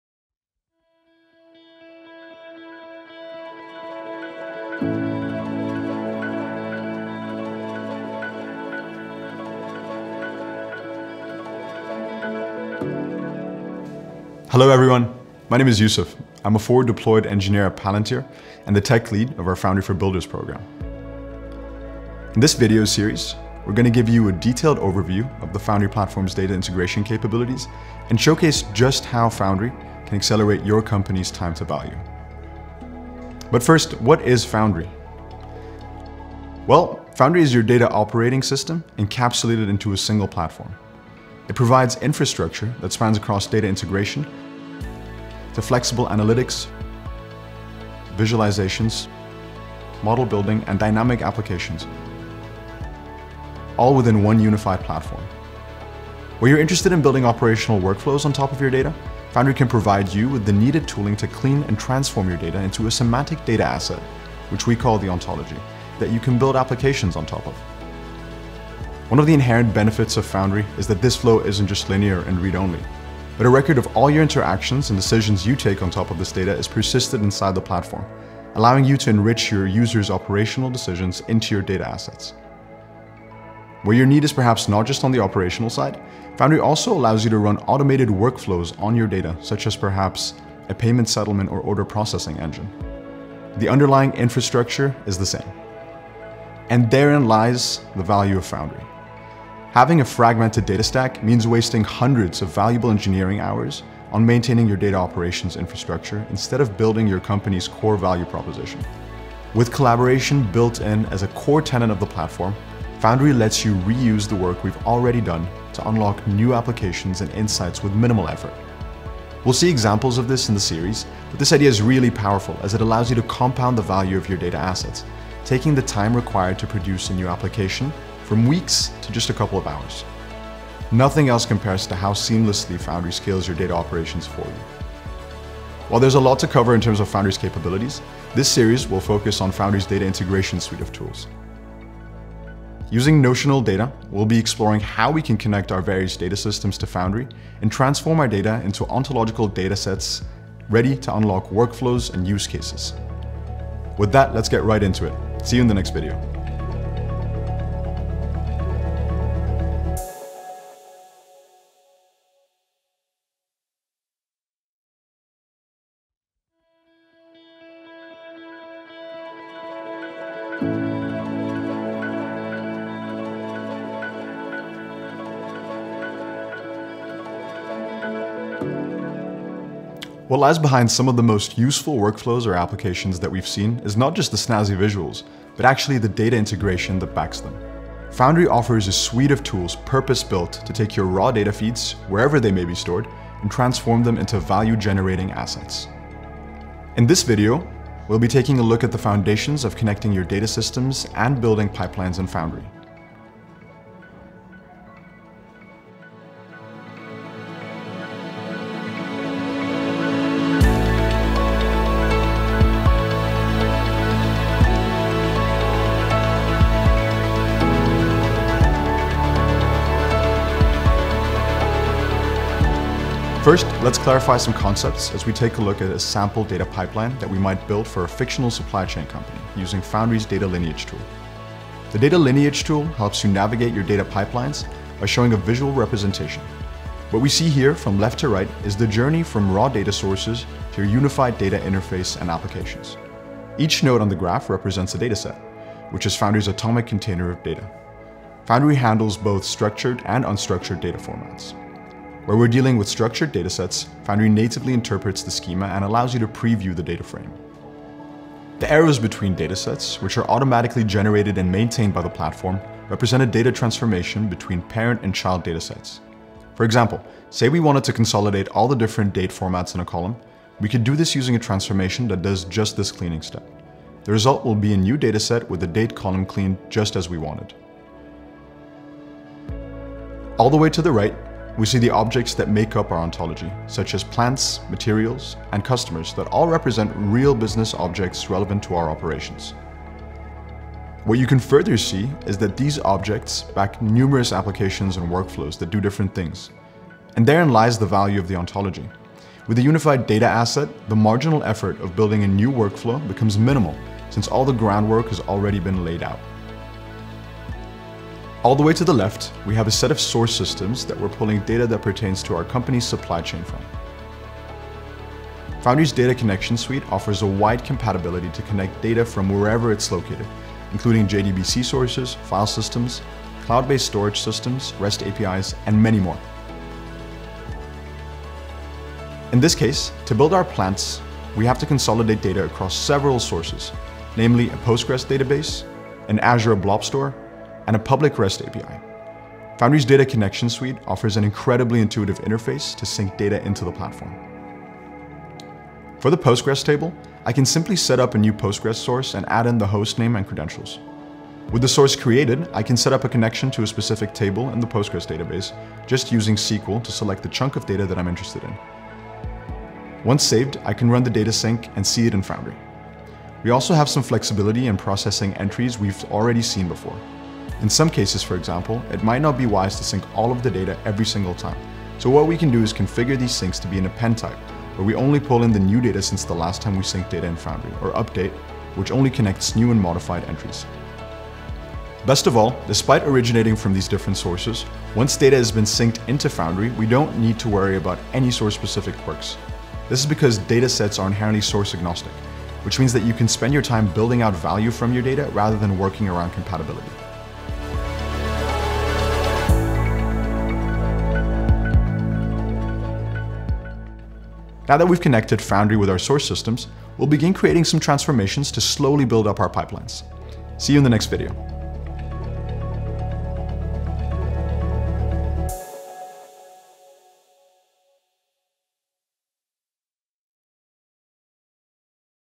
Foundry Tech Playlist | Data Integration Series [Parts 1 and 2]
Parts 1 and 2: Intro to Data Integration + Data Connection

Real-time decision-making begins with secure, unfettered access to all of your data. Parts 1 and 2 of our series provides a detailed overview of the Foundry platform’s data integration capabilities. You’ll also learn how to seamlessly connect your various data sources into a single source of truth using Foundry’s Data Connection suite.